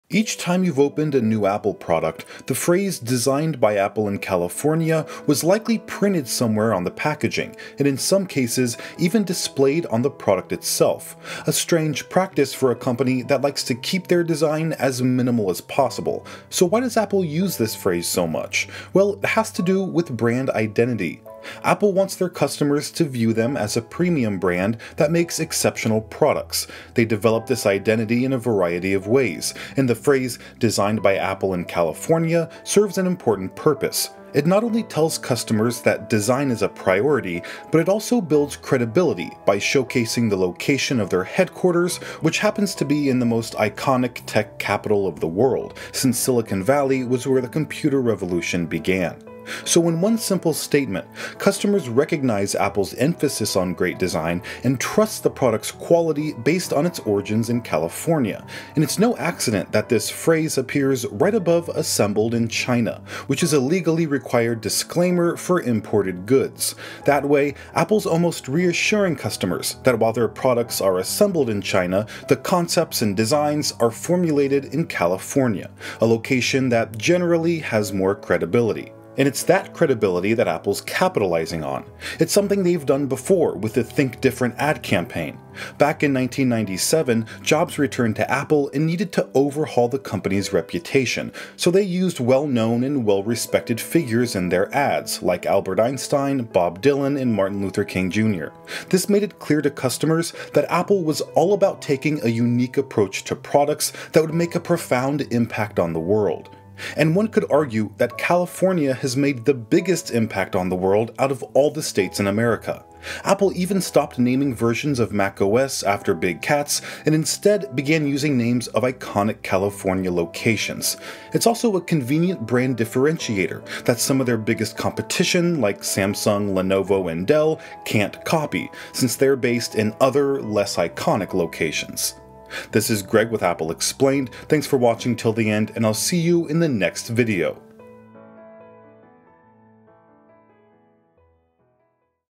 Why Apple Products Say 'Designed in California'
Each time you’ve opened a new Apple product the phrase ‘Designed by Apple in California’ was likely printed somewhere on the packaging, and in some cases, even displayed on the product itself. A strange practice for a company that likes to keep their design as minimal as possible. So why does Apple use this phrase so much? Well, it has to do with brand identity.